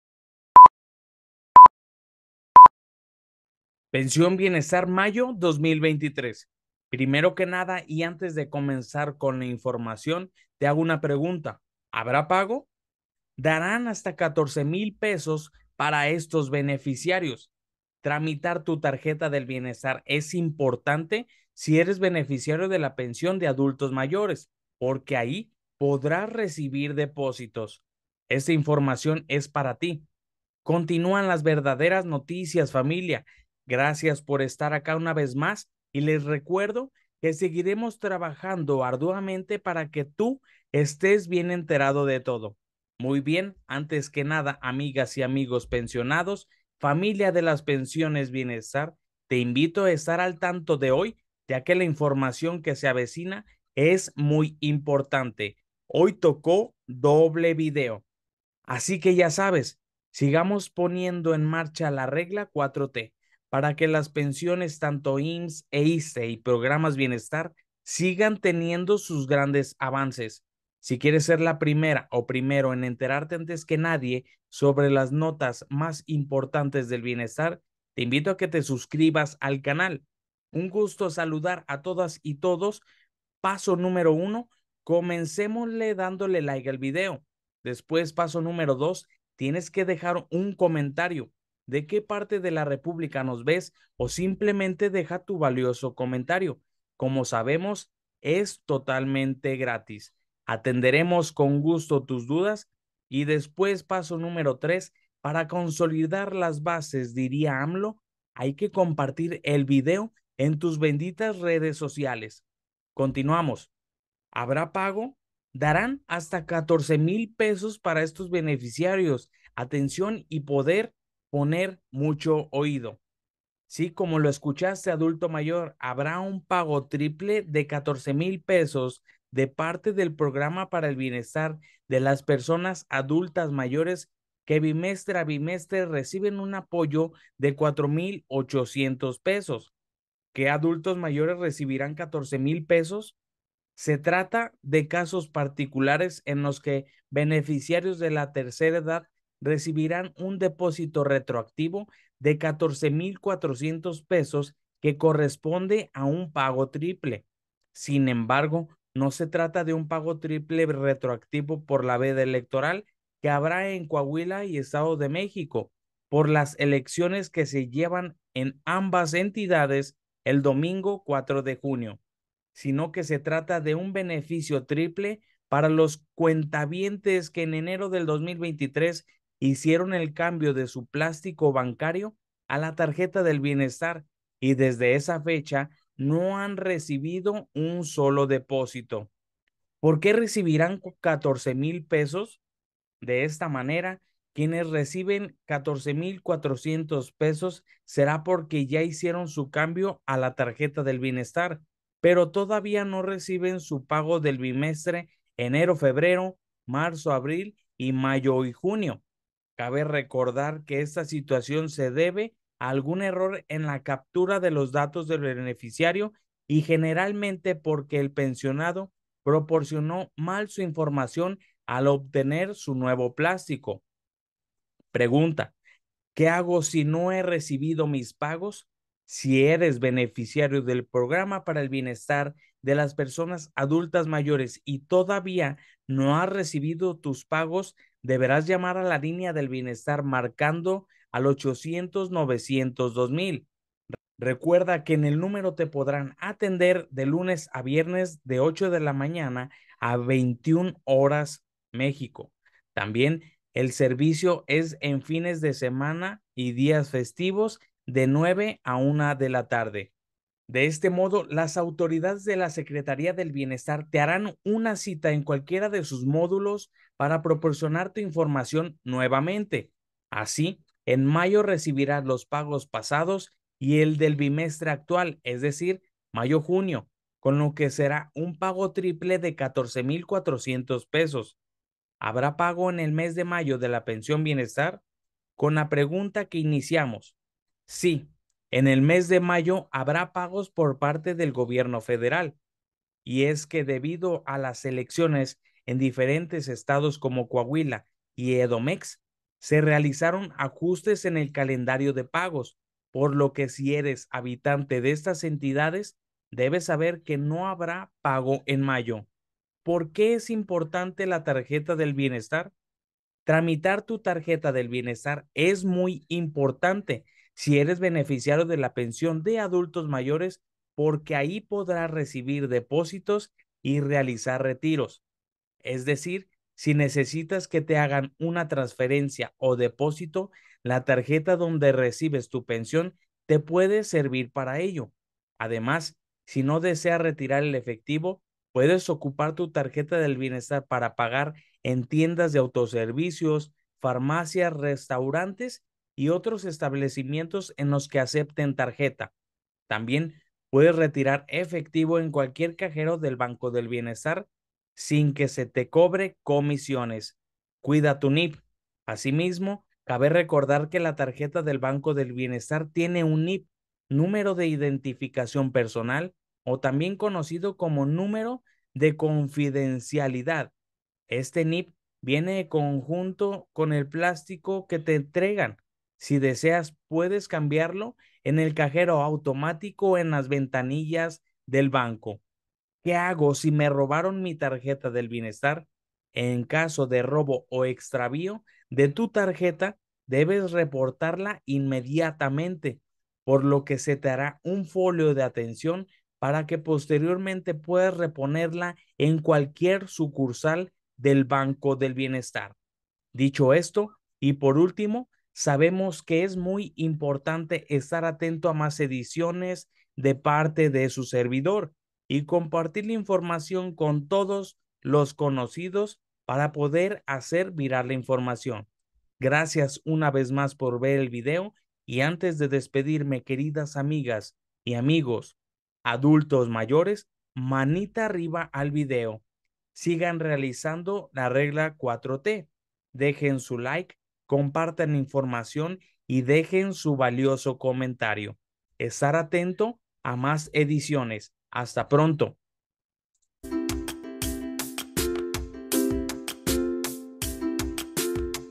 🔥HABRÁ PAGO ❓ADULTOS MAYORES NOTICIÓN BIENESTAR DEPOSITO PENSION POR MÁS DE 14 MIL PESOS💸ENTERATE
El Programa para el Bienestar de las Personas Adultas Mayores anuncia un pago triple de hasta 14 mil pesos para algunos beneficiarios. Te contamos los detalles de quiénes recibirán este beneficio y por qué. Además, te explicamos la importancia de la Tarjeta del Bienestar para recibir y utilizar tu pensión.